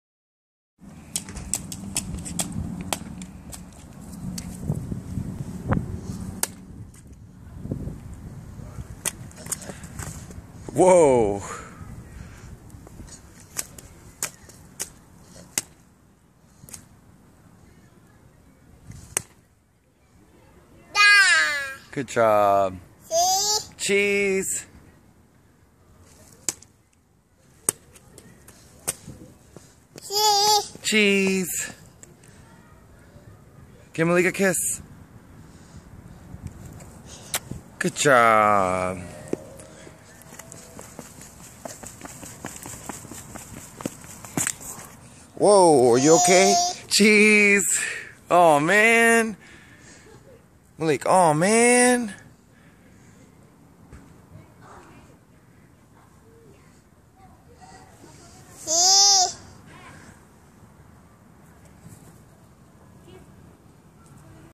Malik Williams "King Malik"
Hanging out with my nephew Malik outside. He trips and falls, then tries to play if off saying "Cheeeeese" to the camera.